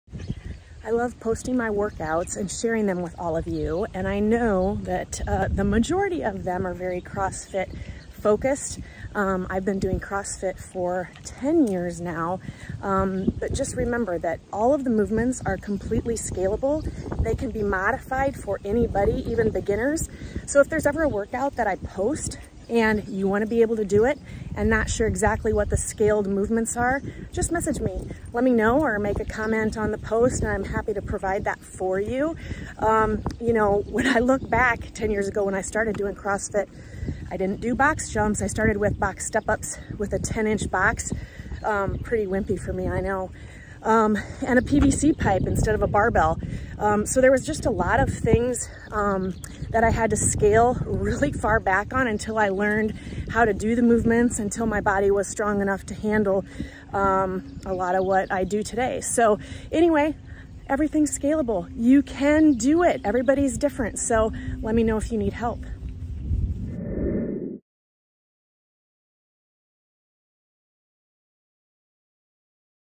Every Movement can be Scaled. You CAN Do it!
The workouts I post are meant to inspire you to get up and move and do something good for your body. Every movement is scalable. Just because it is CrossFit based do not be intimidated or scared. There are ways to modify every work out so even a beginner￼ can do them. Workout should be fun and a way to celebrate what your body can do. At the end it should be a well-deserved accomplishment￼!